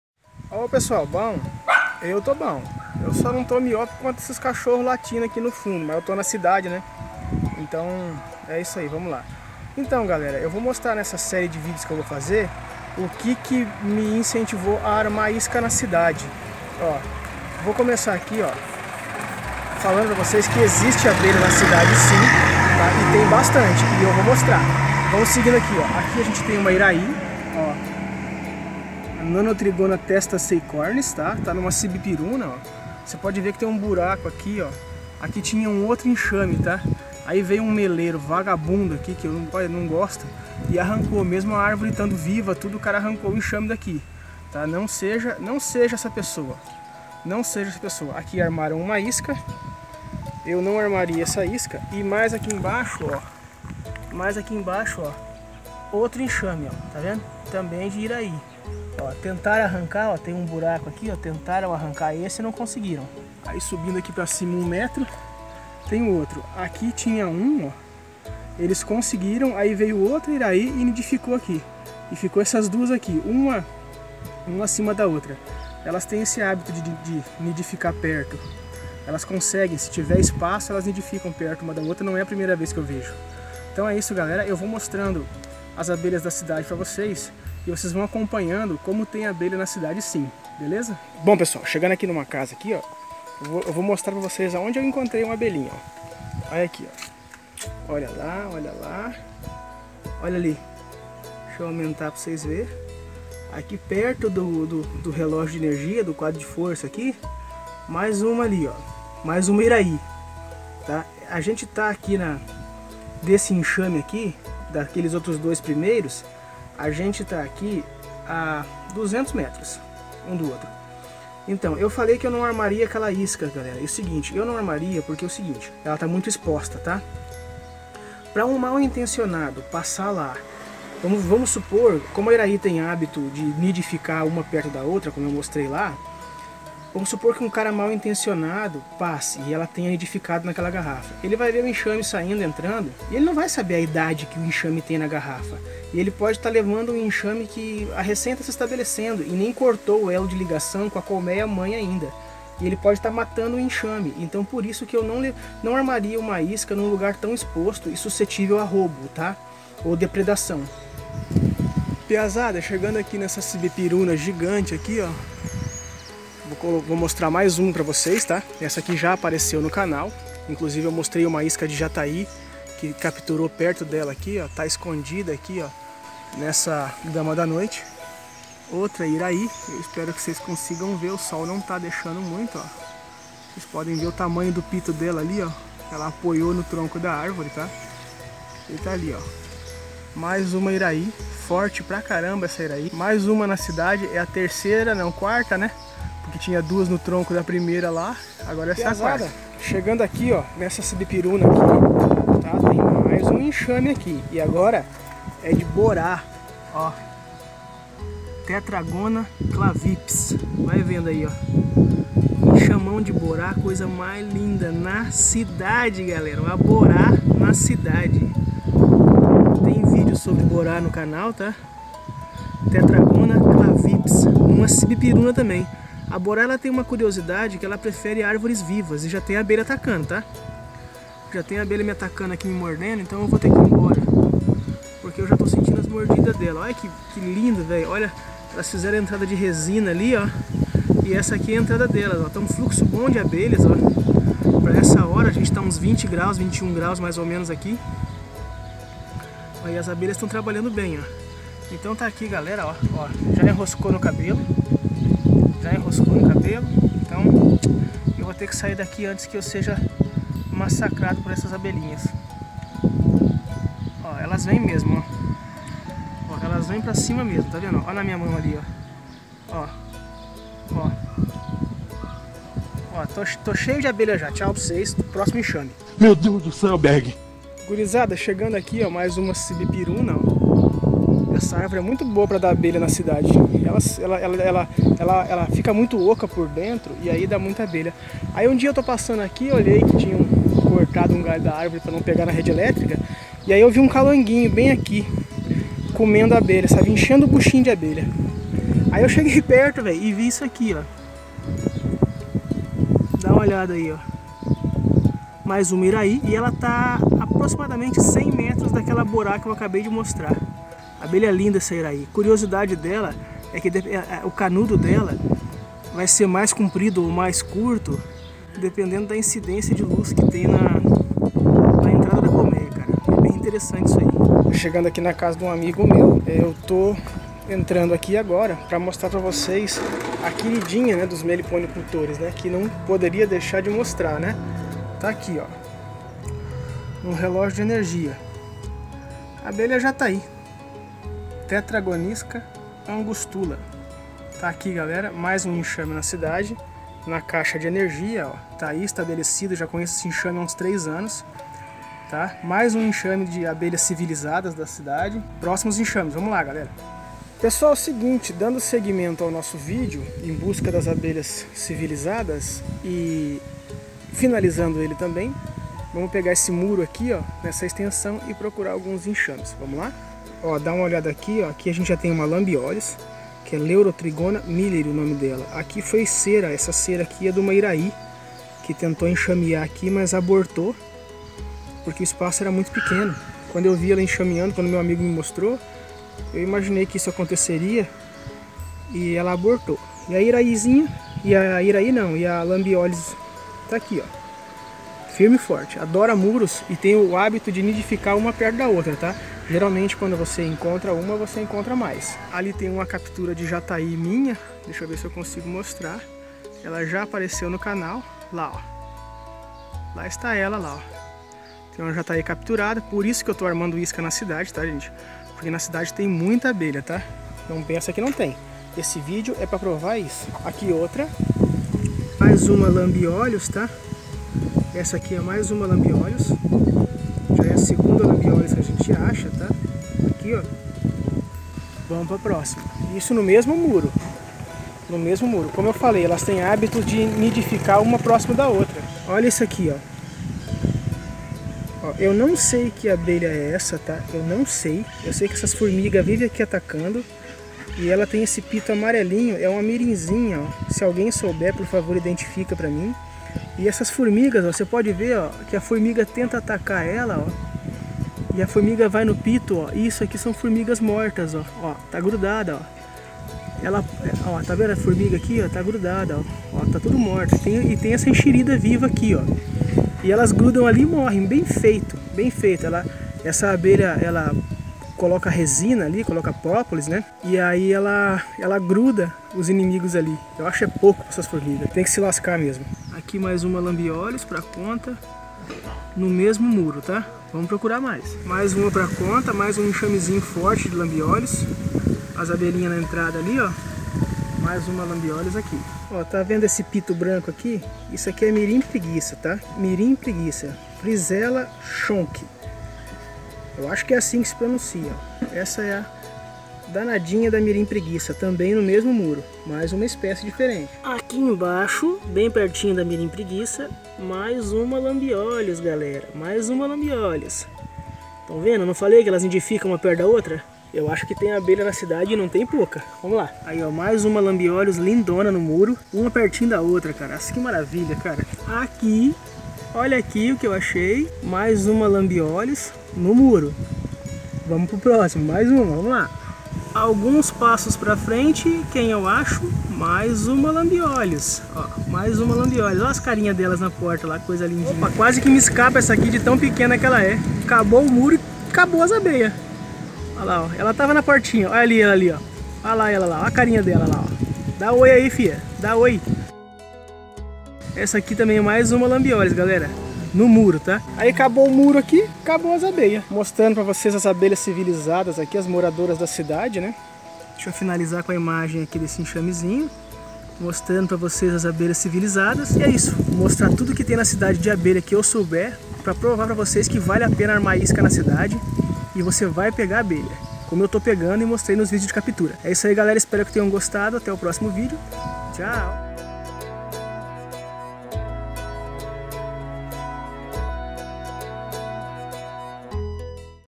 Abelhas sem ferrão na cidade, existem? (Vídeo 1 - Abelhas da cidade)
Olá! Neste vídeo mostro enxames de abelhas sem ferrão(nativas) na cidade, que foi o que me incentivou a começar a armar iscas na cidade.